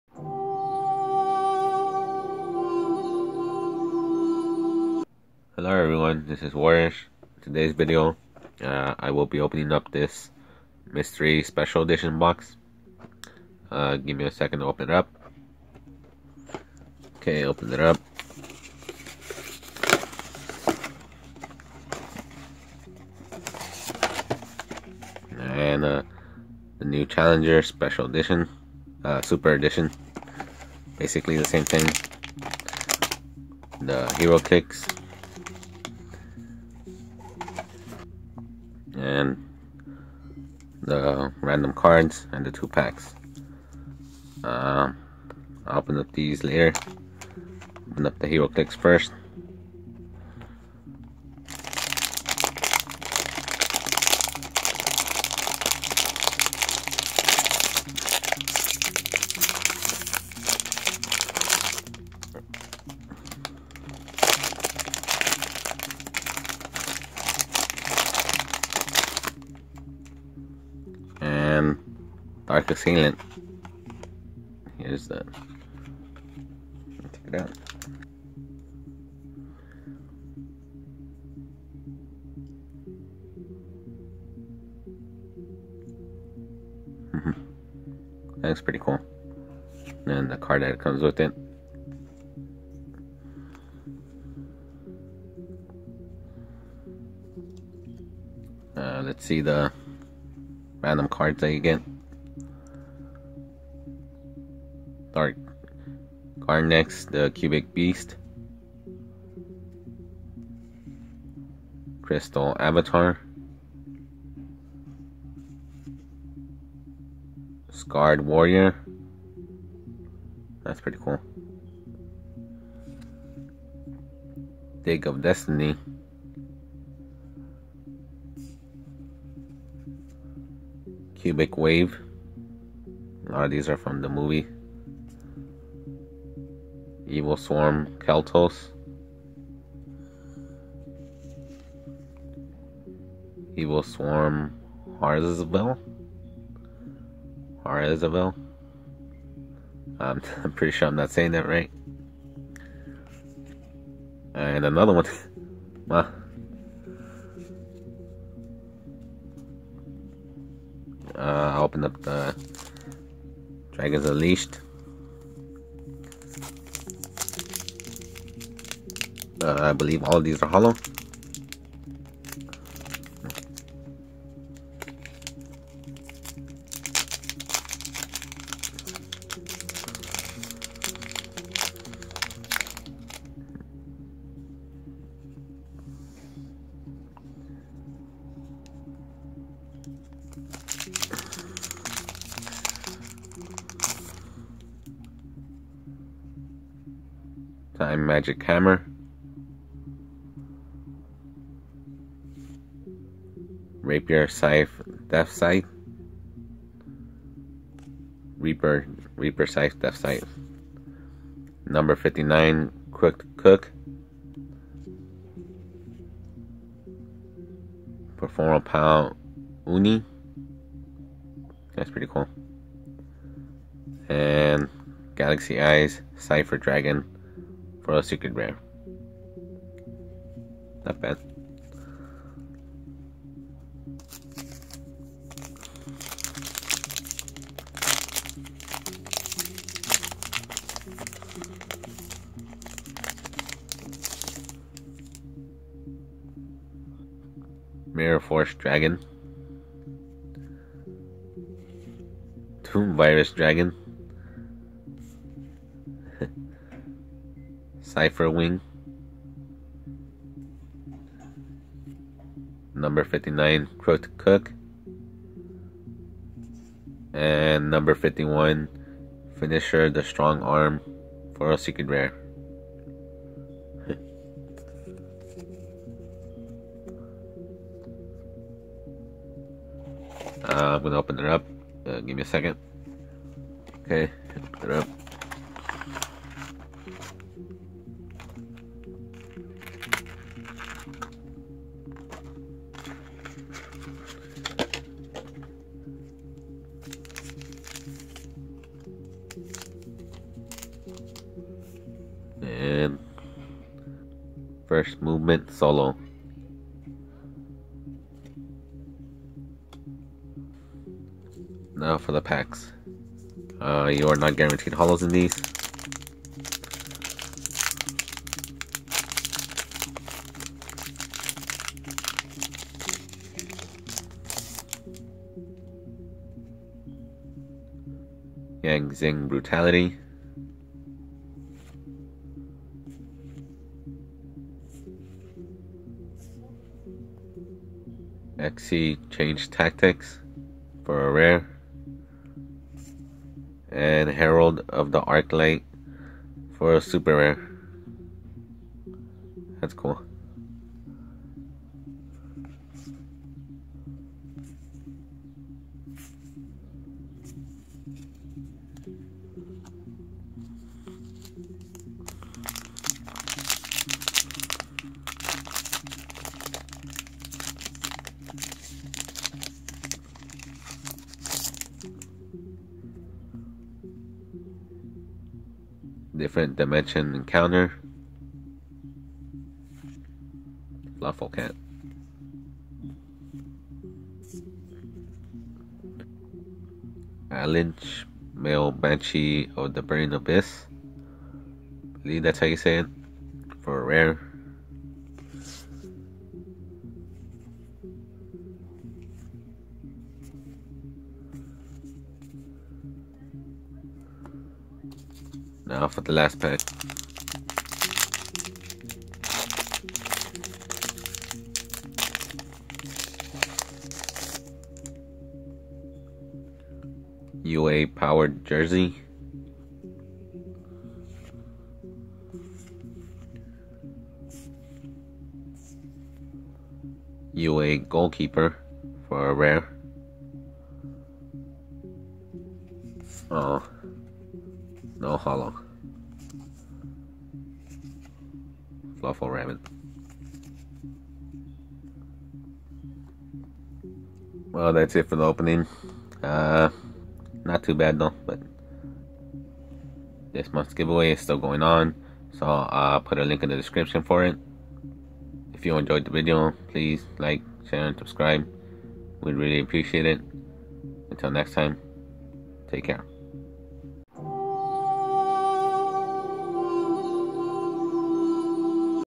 Yu-Gi-Oh! Mystery Special Edition Box opening.
My Yu-Gi-Oh! Mystery Special Edition Box opening. What other openings would you want to see?